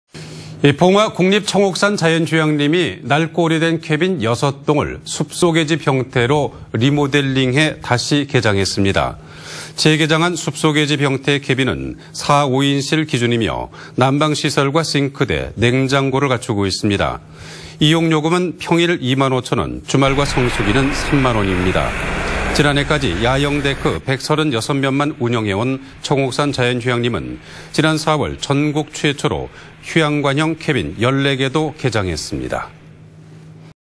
[안동MBC뉴스]청옥산 자연휴양림, '휴양형 캐빈' 운영
2016/06/26 17:45:03    작성자 : 정윤호

봉화 국립청옥산자연휴양림이
낡고 오래된 캐빈 6동을 '숲속의 집' 형태로
리모델링해 다시 개장했습니다.
◀ＥＮＤ▶

재개장한 '숲속의 집' 형태 캐빈은
4,5인실 기준이며, 난방시설과 싱크대,
냉장고를 갖추고 있습니다.

이용요금은 평일 2만5천 원,
주말과 성수기 3만 원입니다.

지난해까지 야영데크 136면만 운영해온
청옥산자연휴양림은, 지난 4월,
전국 최초로 '휴양관형 캐빈' 14개도
개장했습니다.